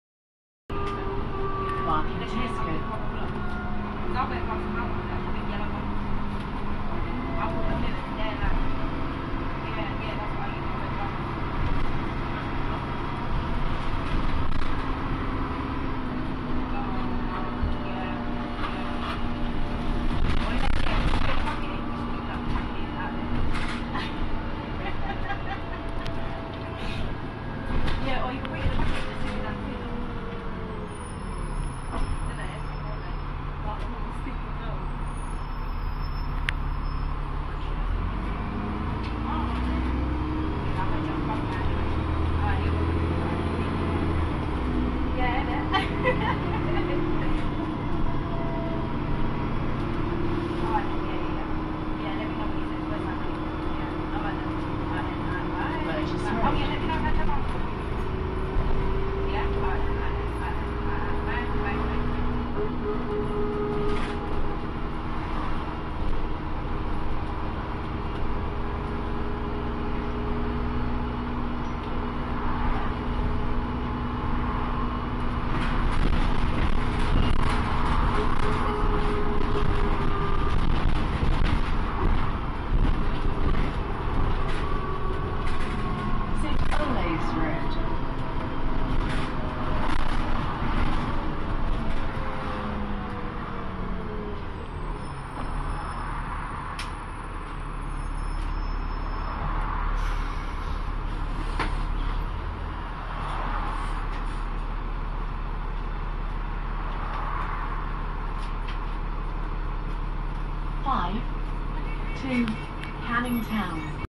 Journey on London Bus Route 5 - 17880 (LX03NGU)
A journey on Stagecoach London's 17880 LX03NGU based at Barking Garage (BK) which is a Euro3 Voith Dennis Trident 2/ALX400. This is on route 5 which operates between Canning Town and Romford Market